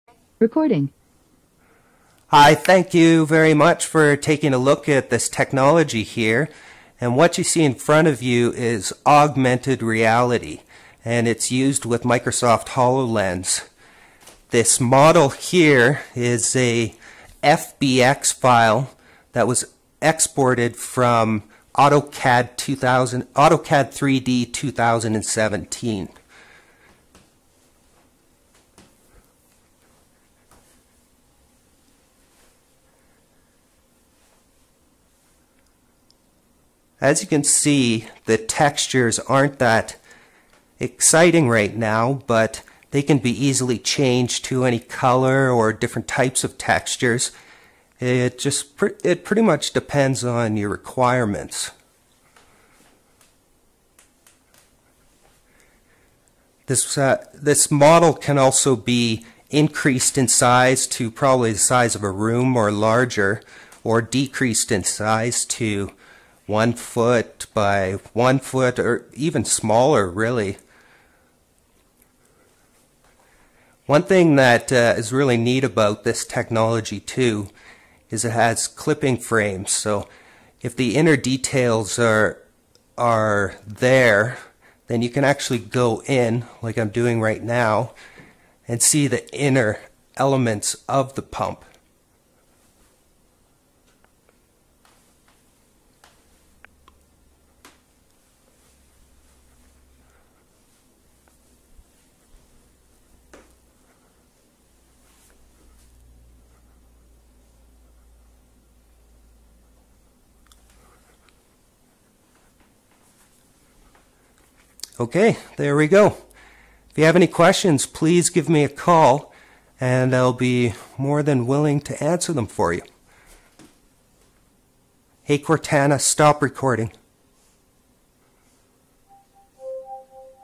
Centrifugal Pump in Augmented Reality
Shared from BizPad Technologies HoloLens.
As mentioned in the video, this is a drawing exported from Autocad 2018 as an FBX file.

Trevor Paddock
BizPad Technologies